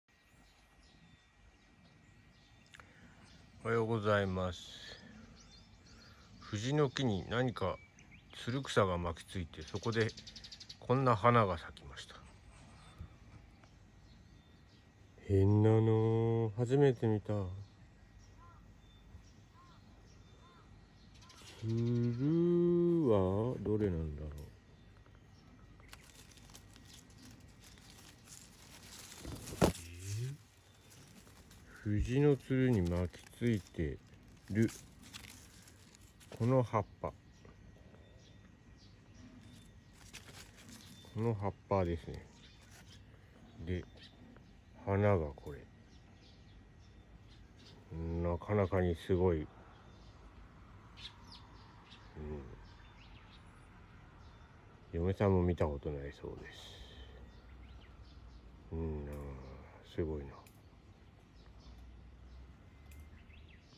不思議な花 初めて見ました
藤に巻き付いた蔓草の花
なんだかわかりません。